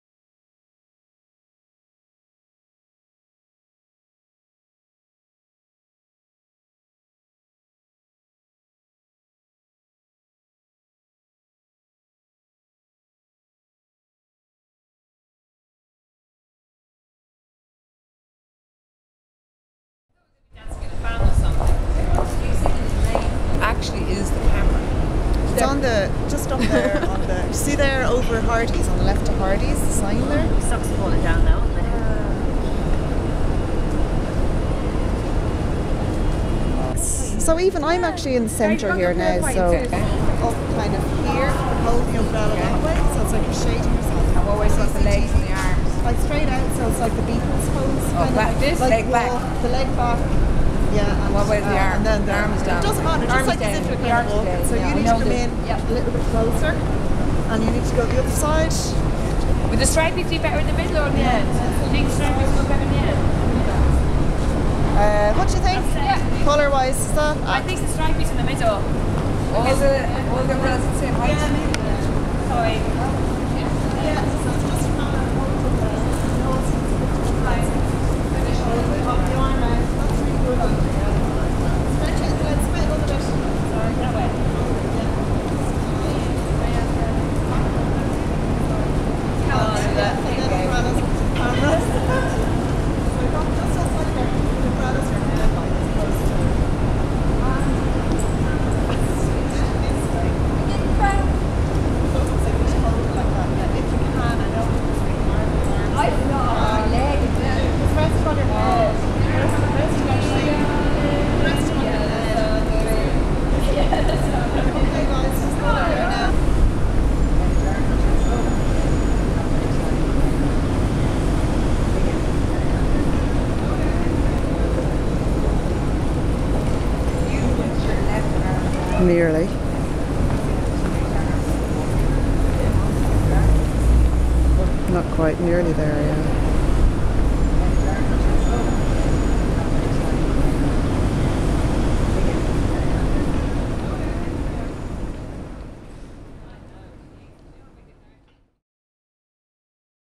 Making of Matthew Street Image
This video is a documentation of the appropriation of an image from a live streamed webcam in Liverpool with a group from NCAD attending the Liverpool Biennial, 2012